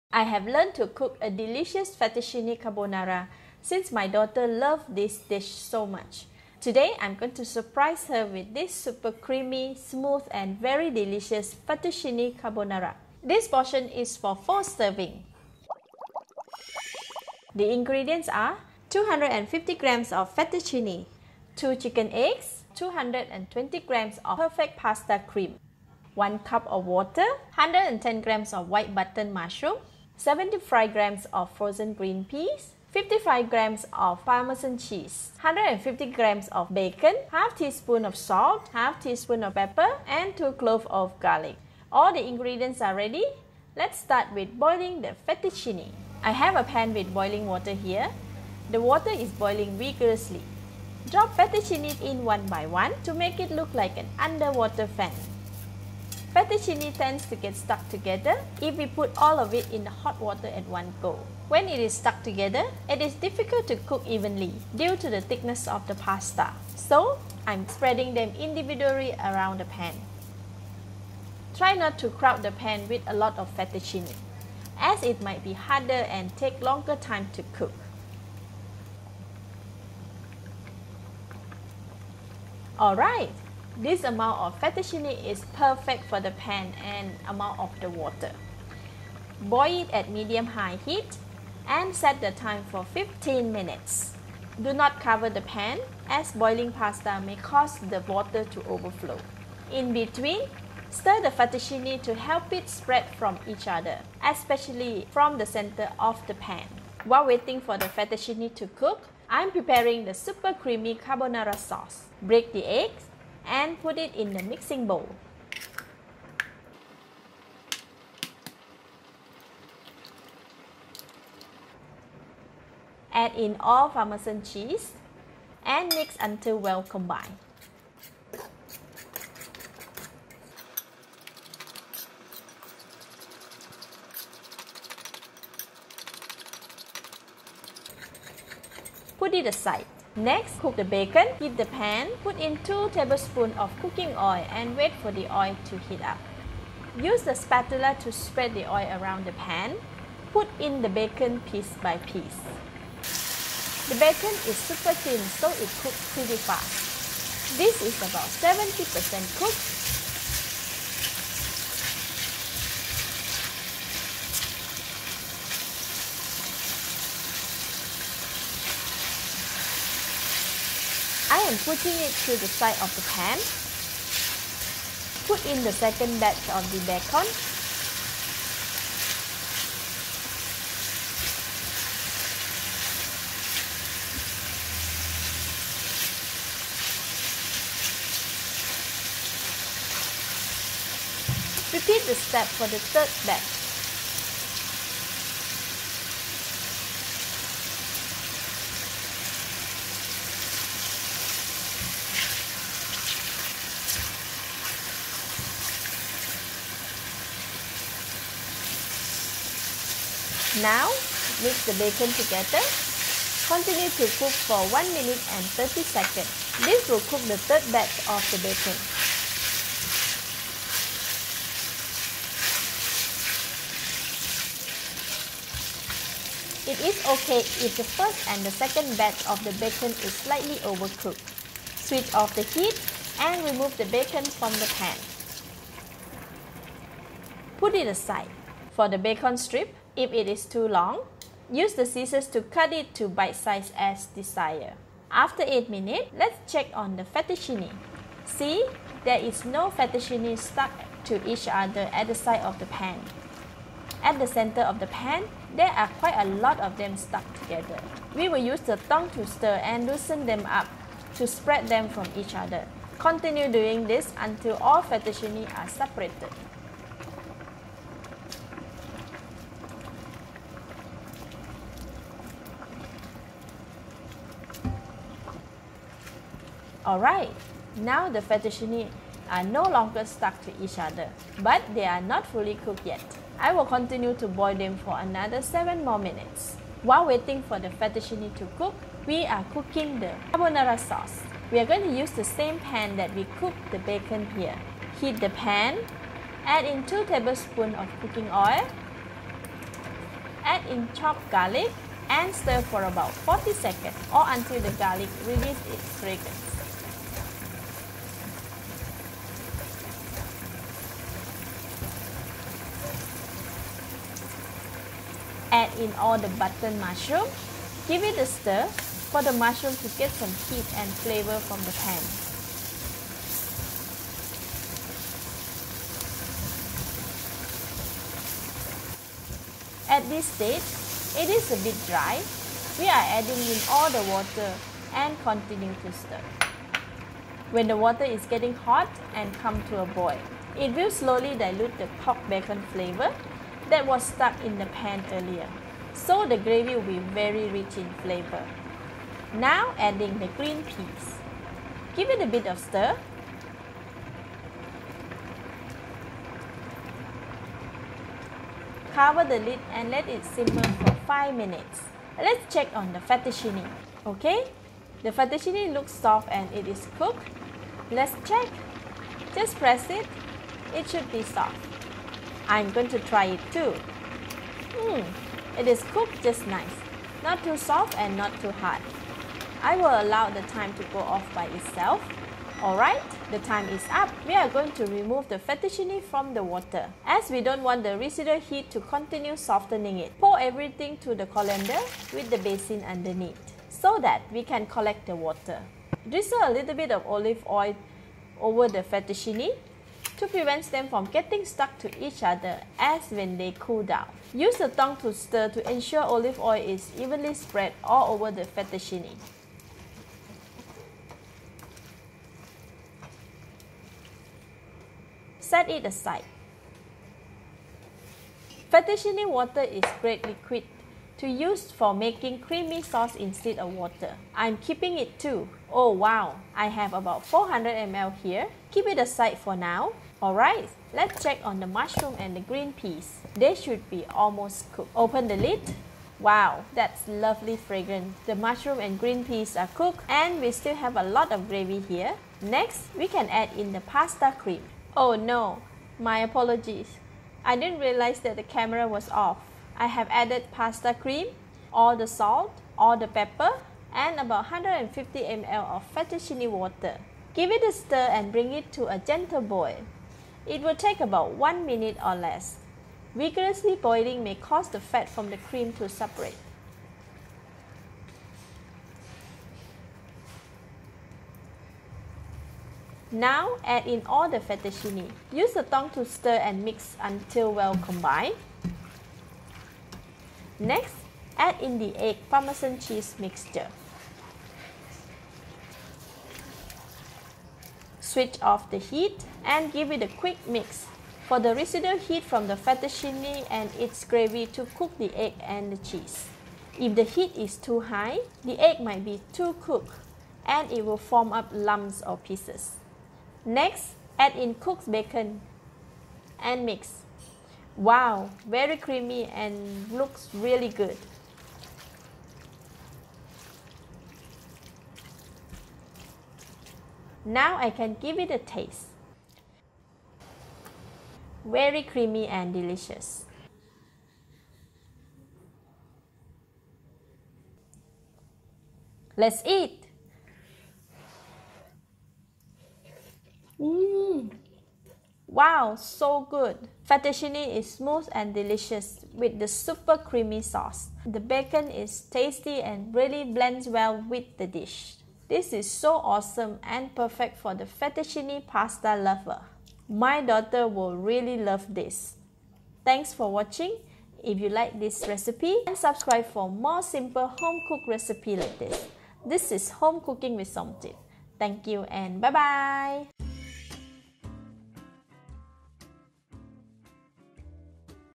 How to Make Super Creamy Fettuccine Carbonara
This Fettuccine Carbonara recipe is a super smooth and creamy. Cooked with bacon and butter, the flavours are bursting with every bite. Enjoy! Ingredients & Utensils ↓

4 Servings

Fettuccine Carbonara Recipe
Ingredients:
1 cup water
1/2 tsp salt